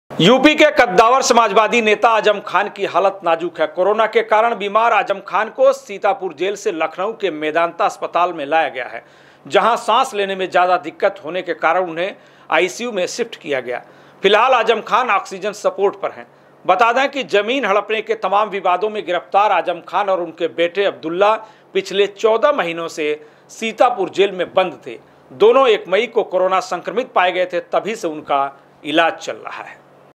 Azam Khan की हालत नाजुक, जेल से शिफ्ट हुए मेदांता अस्पताल
यूपी के कद्दावर समाजवादी नेता आजम खान की हालत नाजुक है. कोरोना के कारण बीमार आजम खान को सीतापुर जेल से लखनऊ के मेदांता अस्पताल लाया गया है,